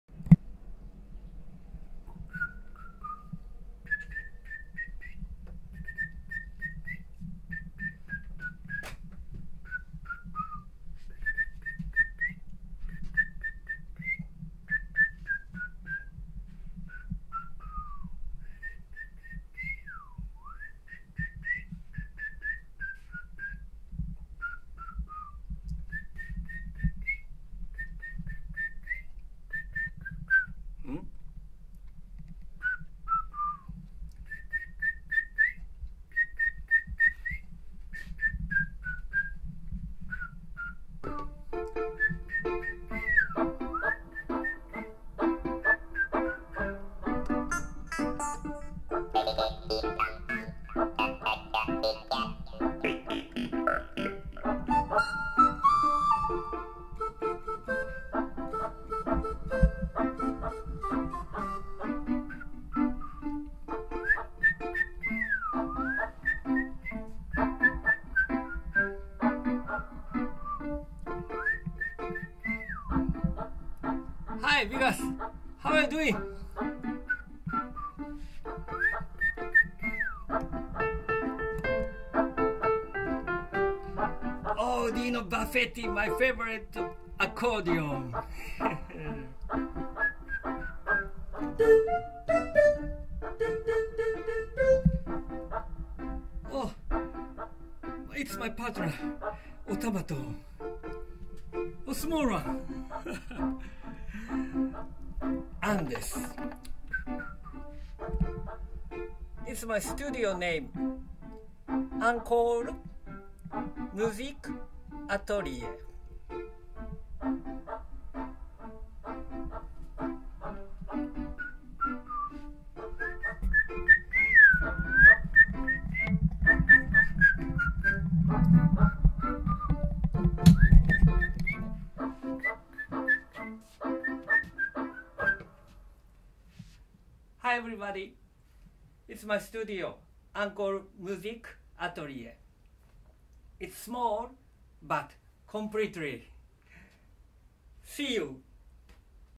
Kunitech Music384”It's my YouTube Studio”
Appearance of my studio "Encore Musique Atelier”.
Commentary is Kunitaka Watanabe
私のスタジオ「アンコール音楽工房」の様子。
解説 渡辺邦孝